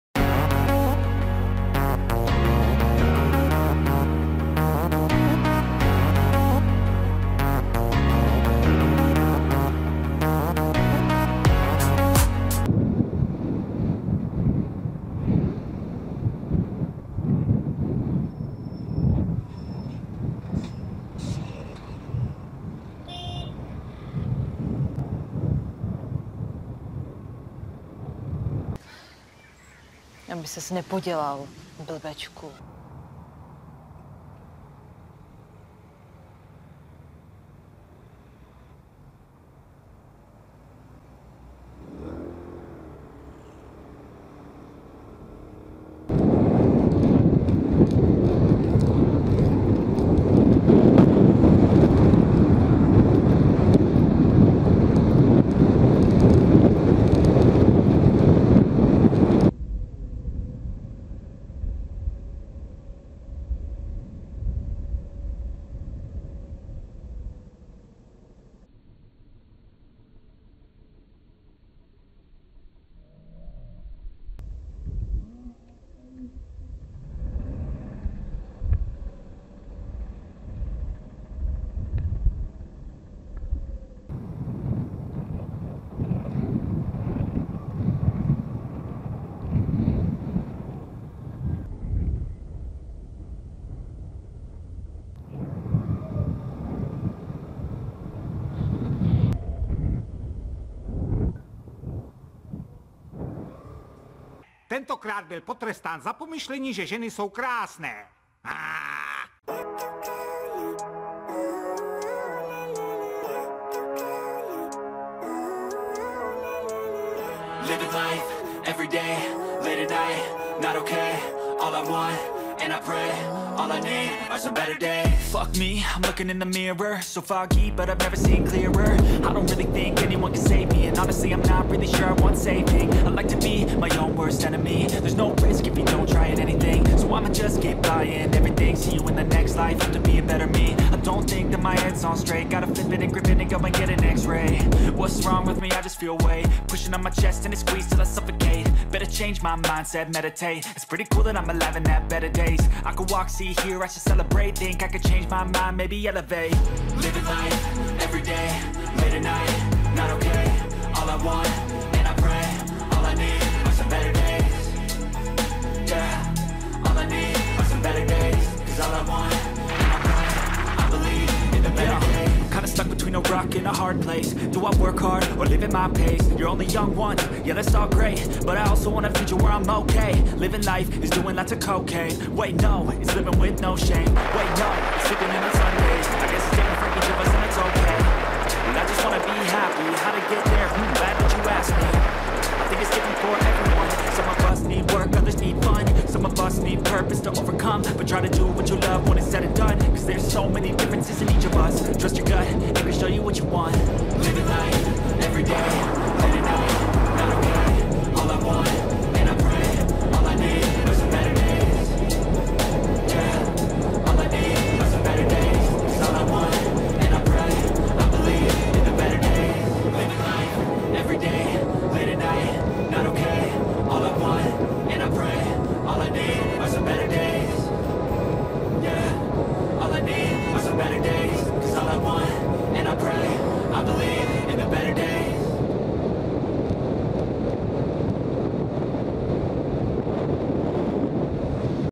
Sražený chlap, Honím 🚦🍆😛, Skoro jsem se 💩 - SkútrCam #52
Dnes uvidíte:
0:00 Intro
0:12 Honím
0:32 Červená
0:45 Neblikač
1:00 Sražený chlap
1:49 (ne)ZIP
2:08 Předjíždění přes zebru
2:15 Vytlačení
2:25 Zdržovač